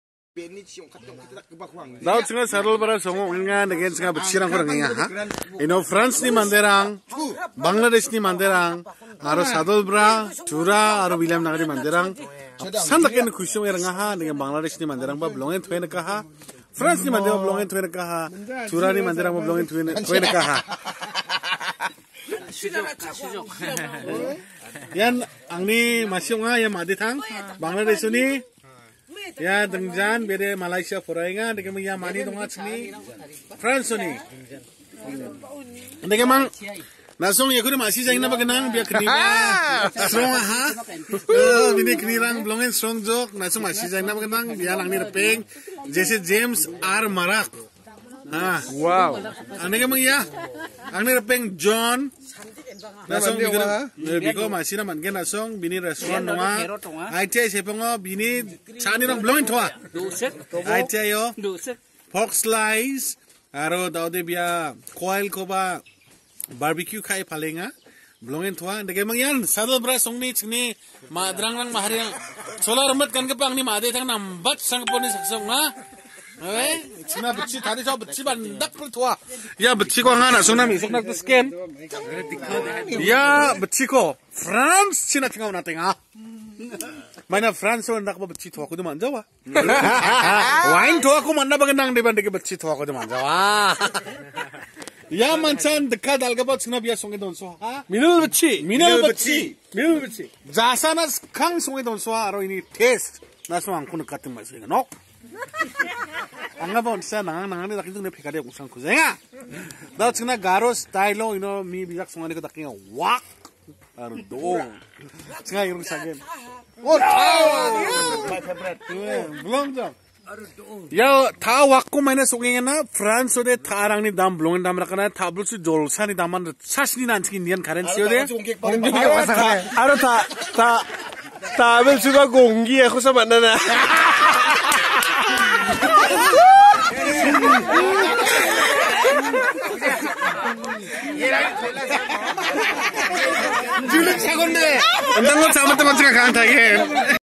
Visit to Sadolbra with friends and relatives from France and Bangladesh
Sadolbra is a typical Garo village in Garo Hills